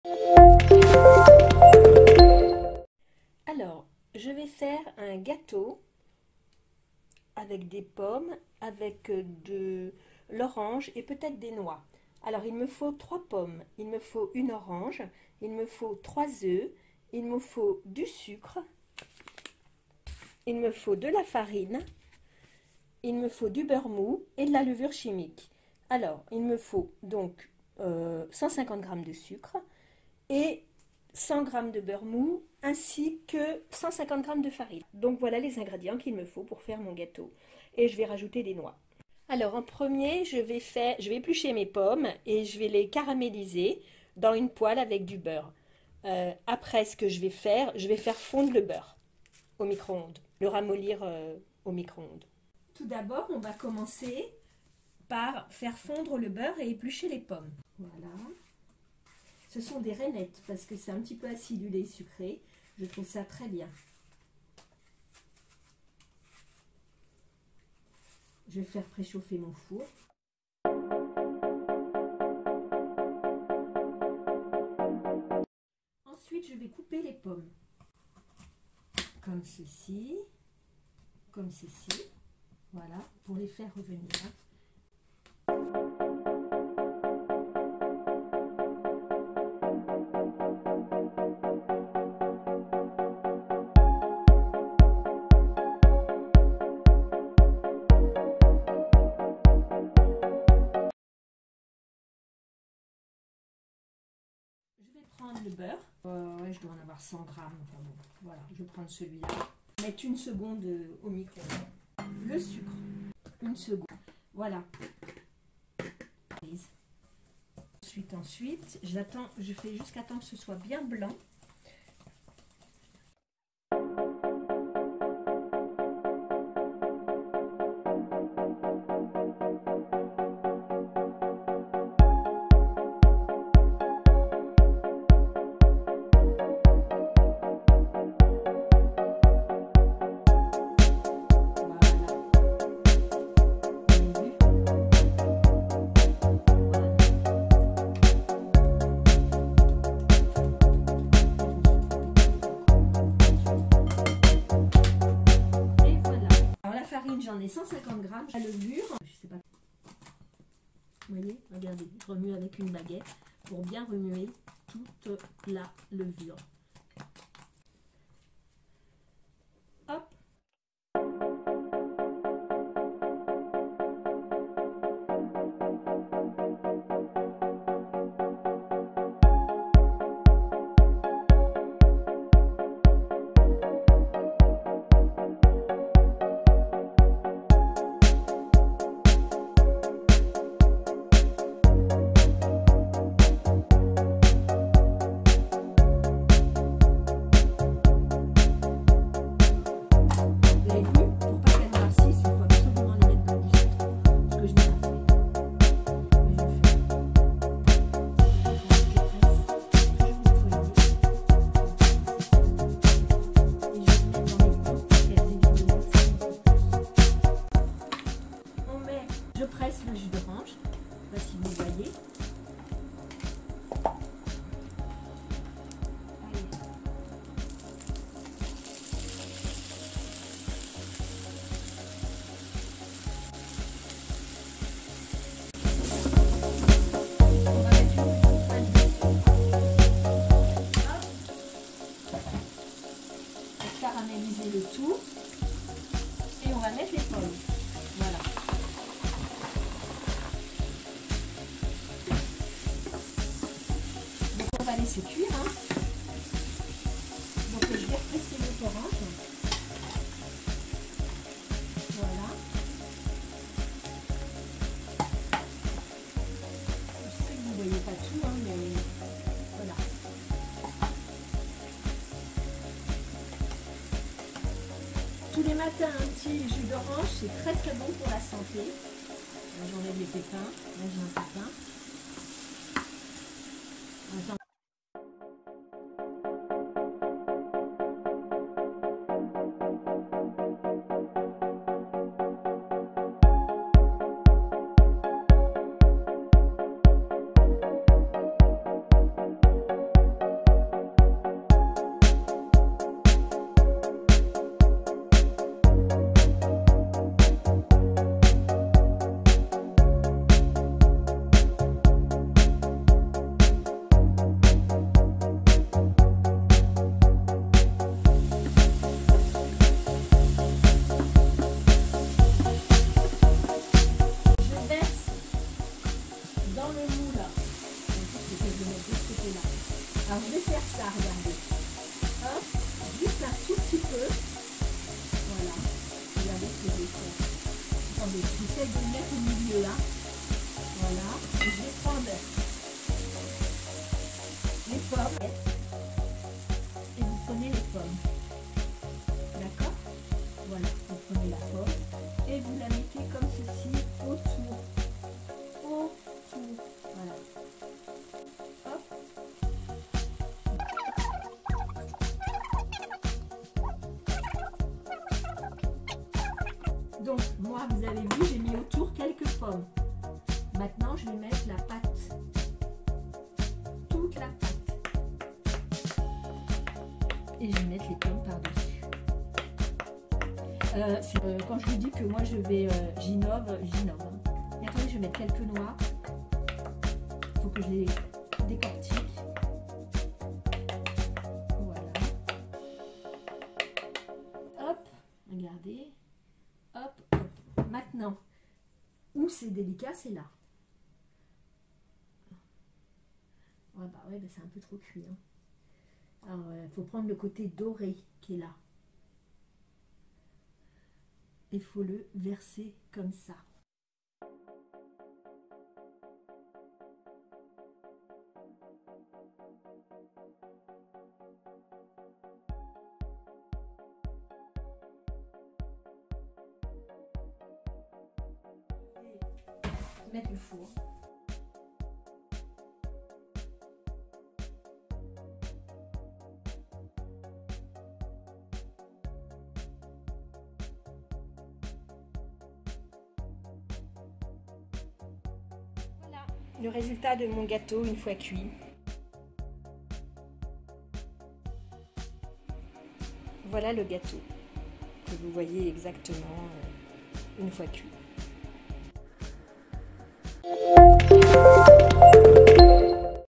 MIAM!: GATEAU AUX POMMES CARAMELISEES AUX JUS D'ORANGE ET CES CERNEAUX DE NOIX
Credit:
Generique: